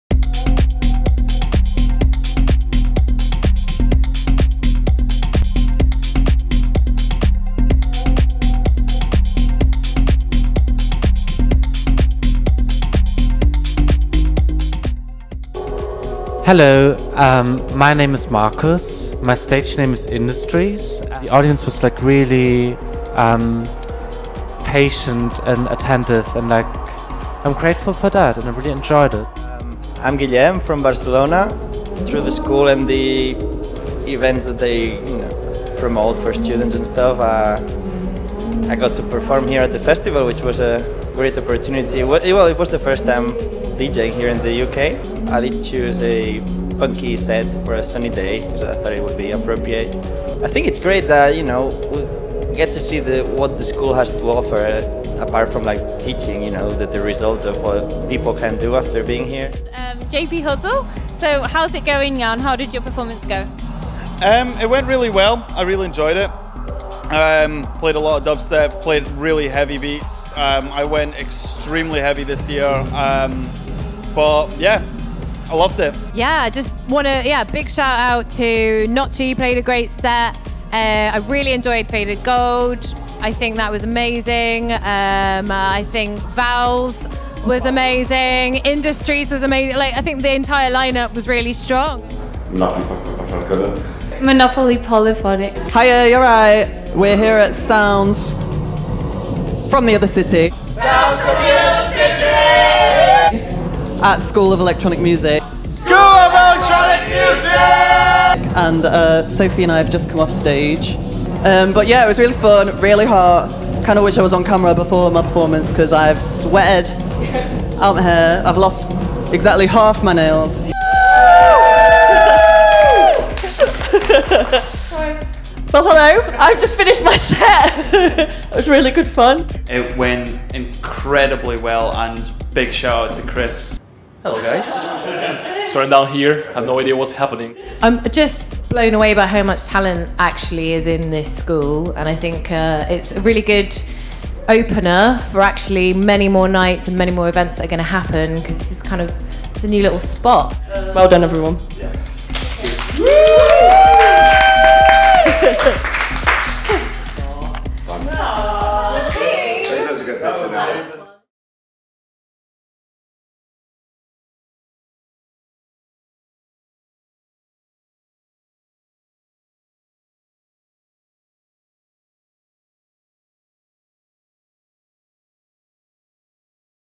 SEM at Sounds from the Other City - May 2018
Just over a week since we took part in Sounds From The Other City festival here in the sunny City of Salford. Have a look at some of the backstage action from the day. Featuring -Industries. Gimi/Somnia, JP Huzzle, Faded Gold, E11E, Don't Be Cruel, Lucy Ironmonger, Monopoly Phonic, Will Judd, Notchyy 😎